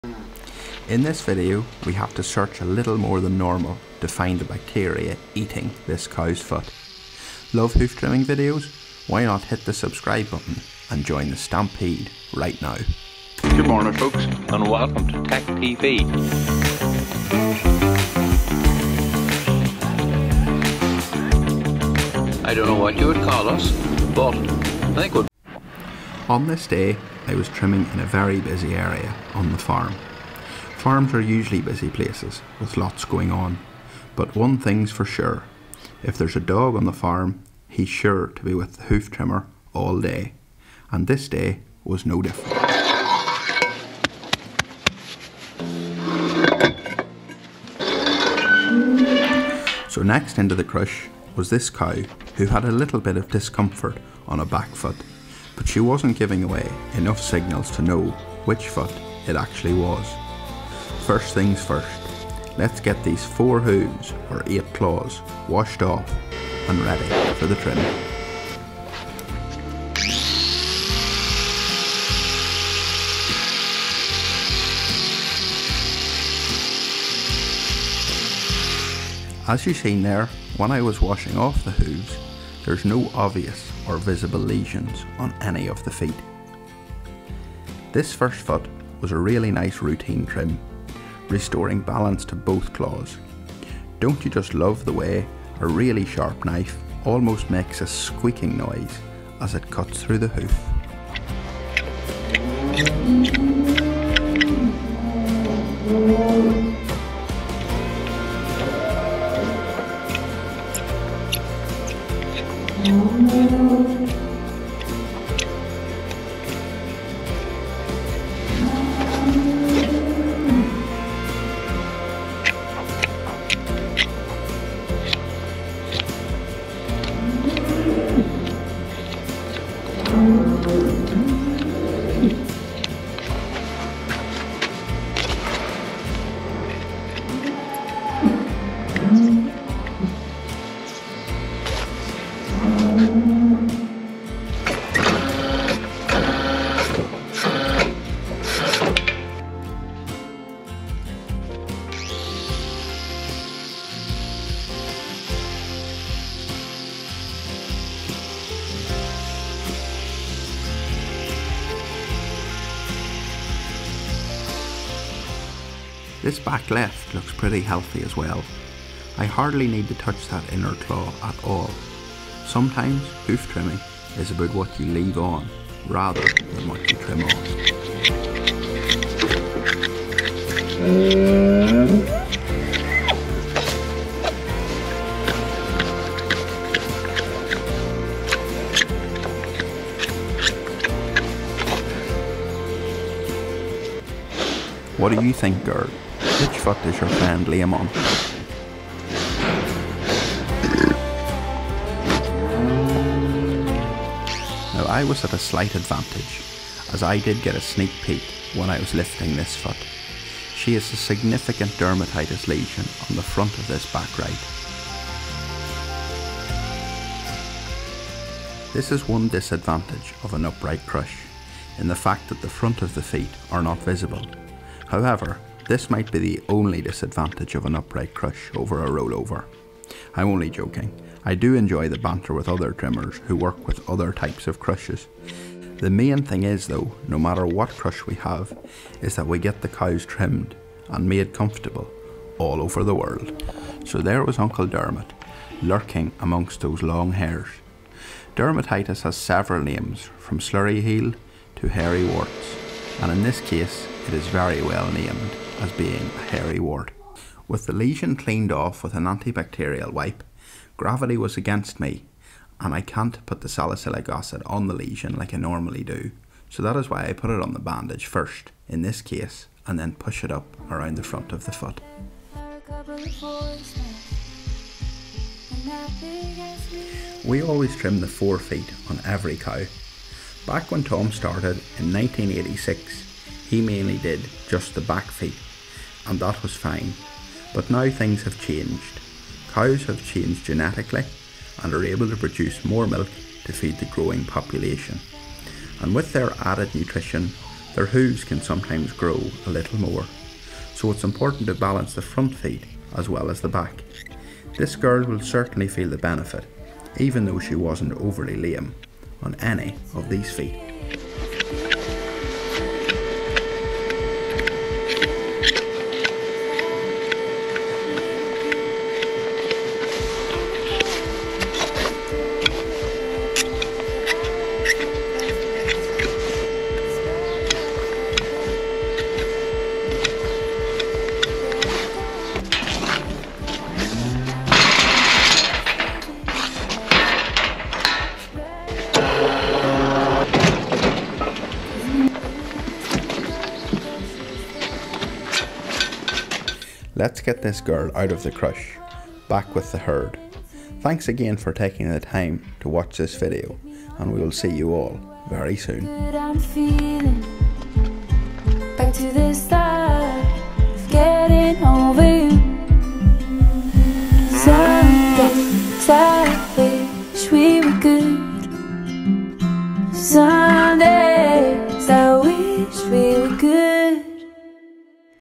BACTERIA LURKING IN COW'S HAIRY WART | TEC TV
In this video Gareth, Professional Hoofcare Specialist is back visiting for a routine trim on a local farm this cow has a hidden problem. Stemming from the top of the heel and infectious disease working round to the front of the hoof. Gareth starts this cow on its journey to full recovery and teaches us a few things along the way...

We appreciate all your support on this channel but if you want to keep us fuelled up on the road you can now buy us a coffee.